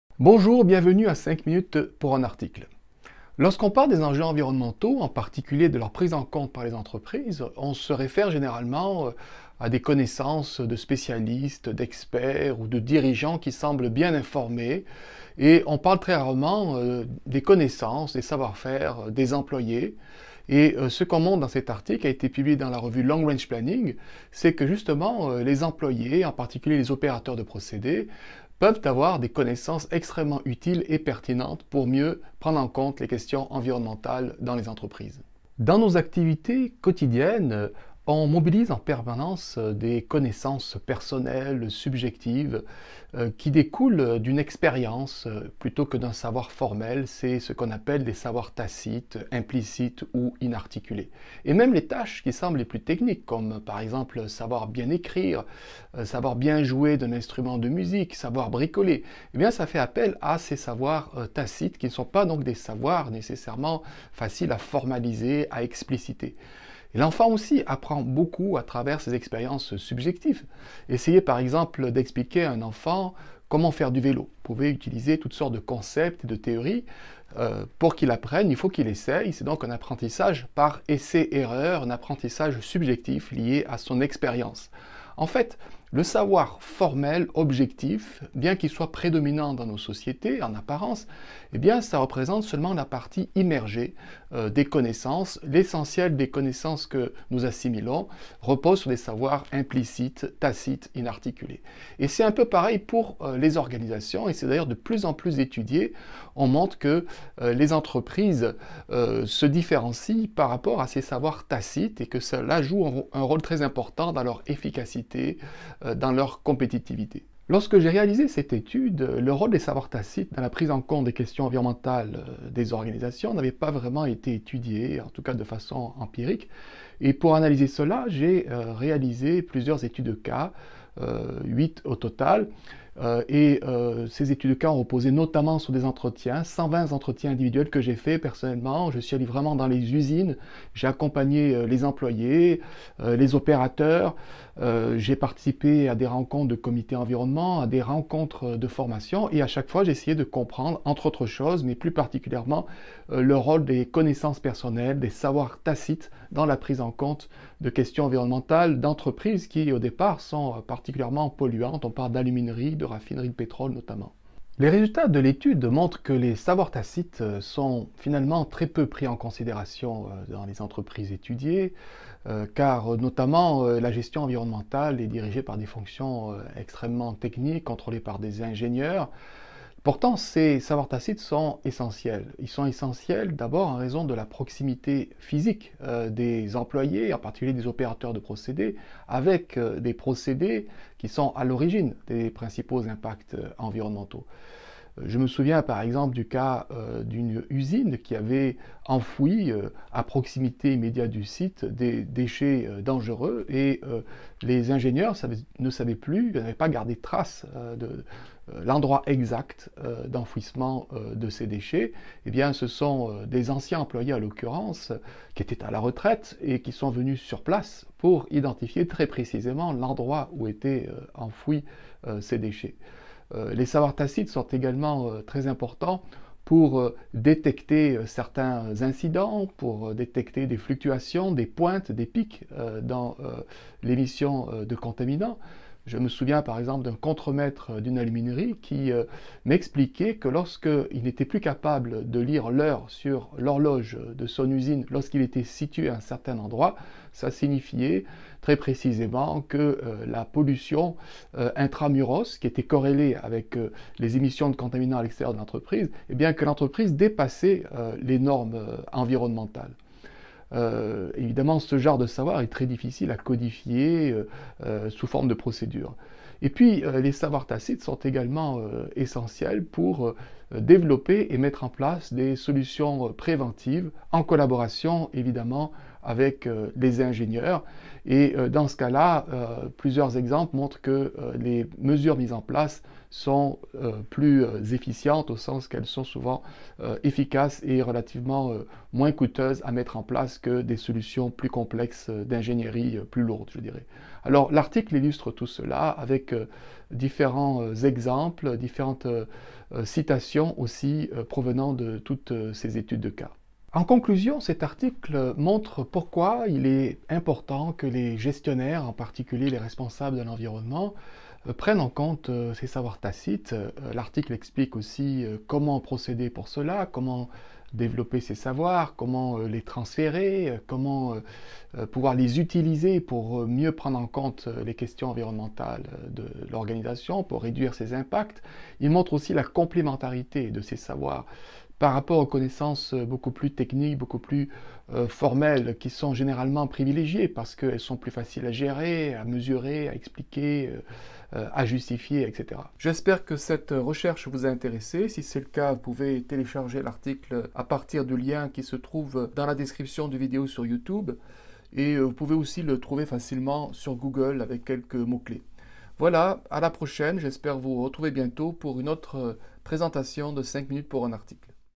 Tacit knowledge and environmental management
Reference: Boiral, O. (2002). “Tacit knowledge and environmental management”, Long Range Planning, 35(3), 291-317.

Abstract
This article demonstrates tacit knowledge’s relevance in environmental management and explores how organisations can manage this knowledge. Through case studies it reveals how taking the tacit knowledge of employees into account can be particularly useful in three key areas of environmental management: the identification of pollution sources, the management of emergency situations and the development of preventive solutions. In order to take tacit environmental knowledge into account, firms must challenge the predominance of formal knowledge in the management of environmental problems and promote a climate of learning that encourages the recognition and sharing of employees’ experiences. The paper also presents a framework for the analysis of the creation, transfer and retention of tacit knowledge that is not limited to environmental knowledge management.